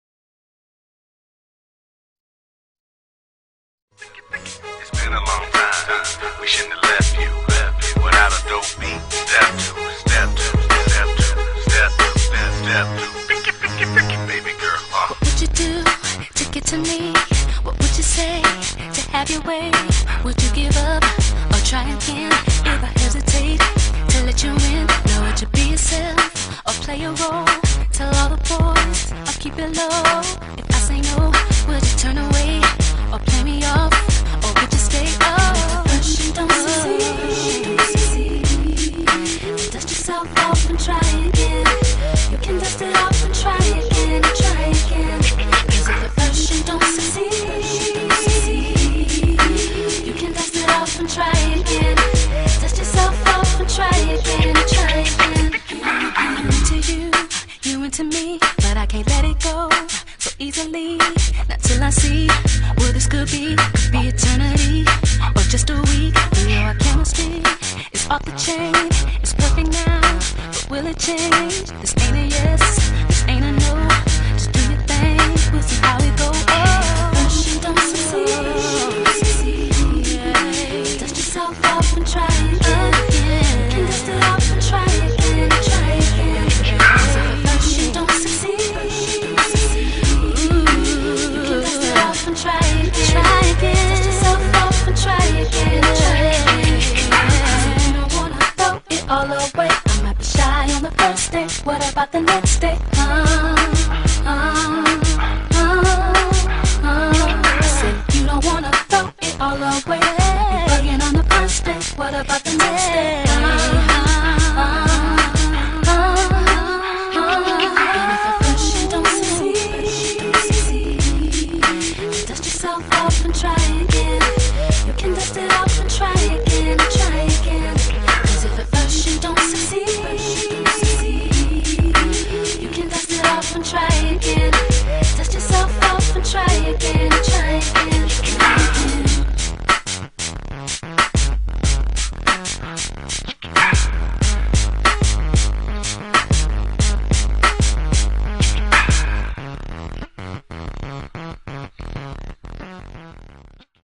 Kacey and Dalton in Try again
This is my friend Kacey and her horse Dalton.